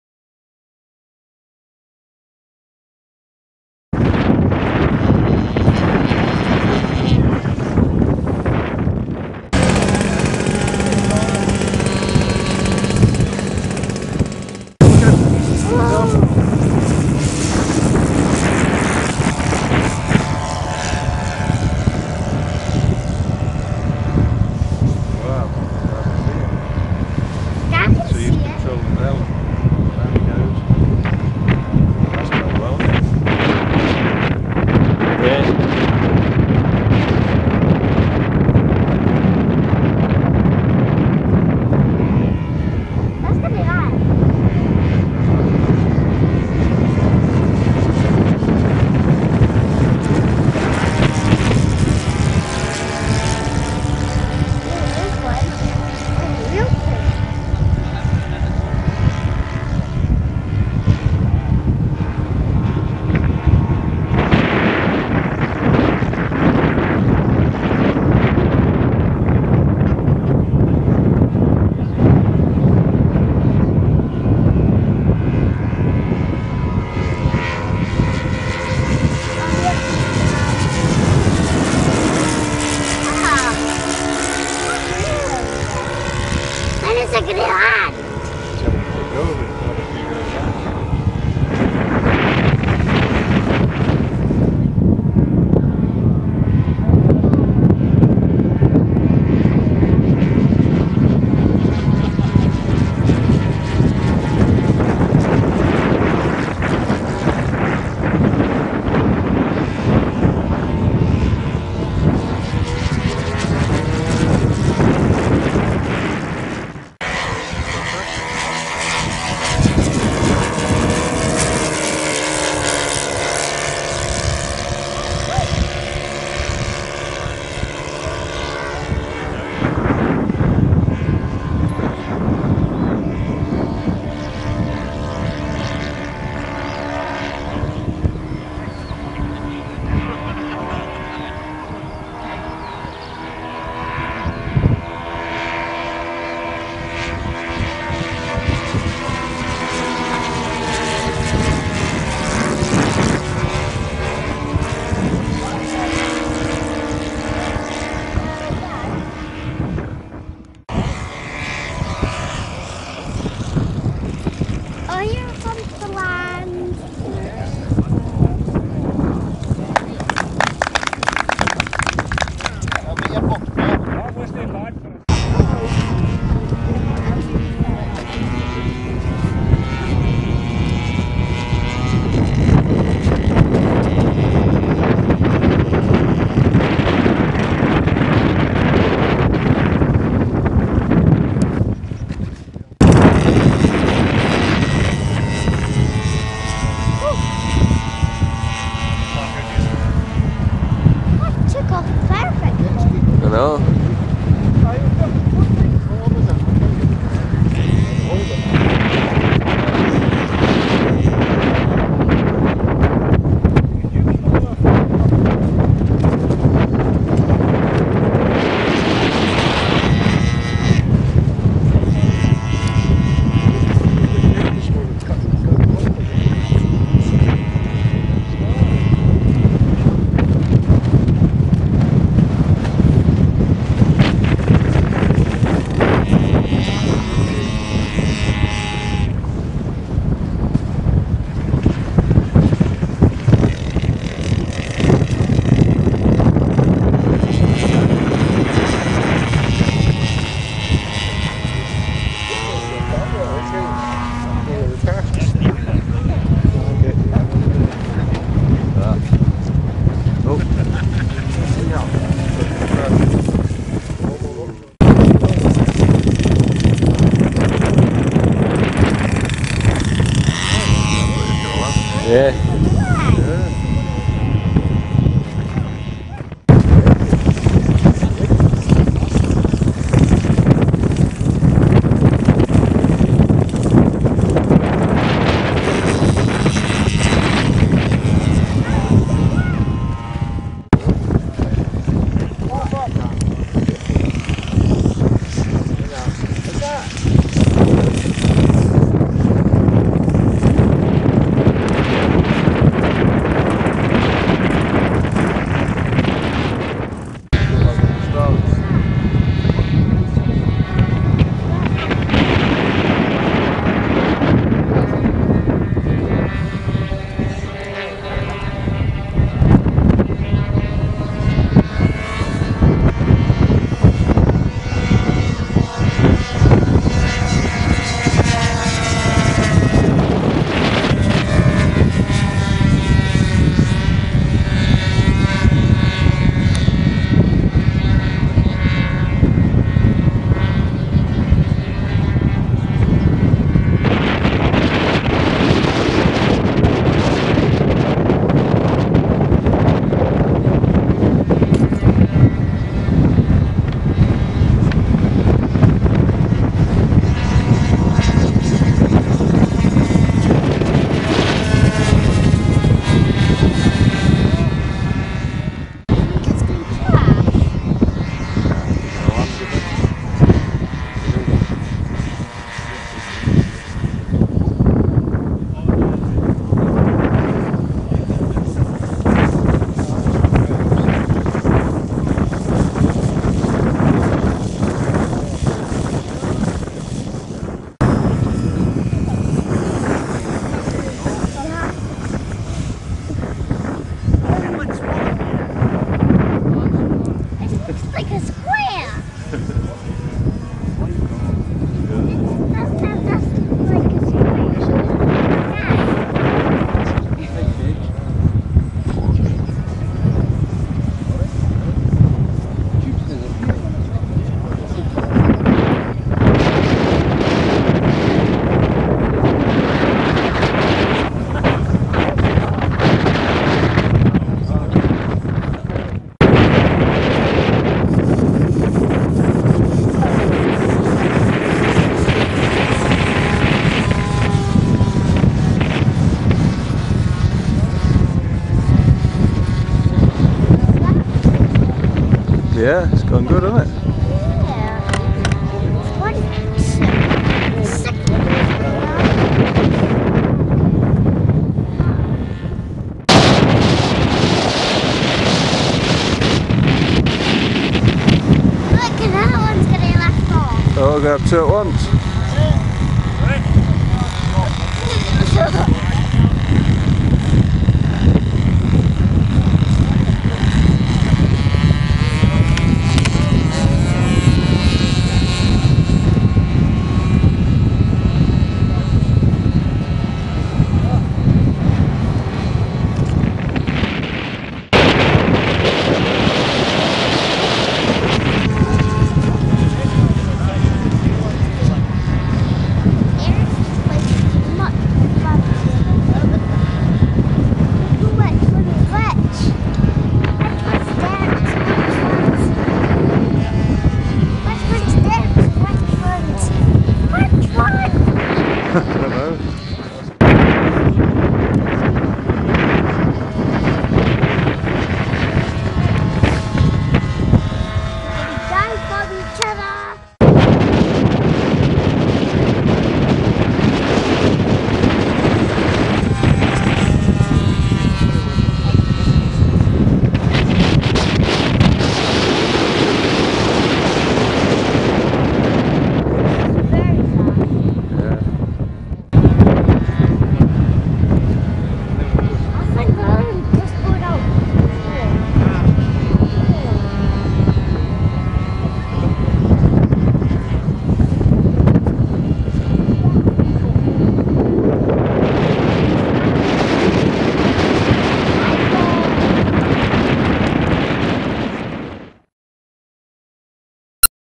FMFC Falkirk Model Flying Club 01052011 part1
1st May fly-in.
Sorry about the wind noise - windy day!!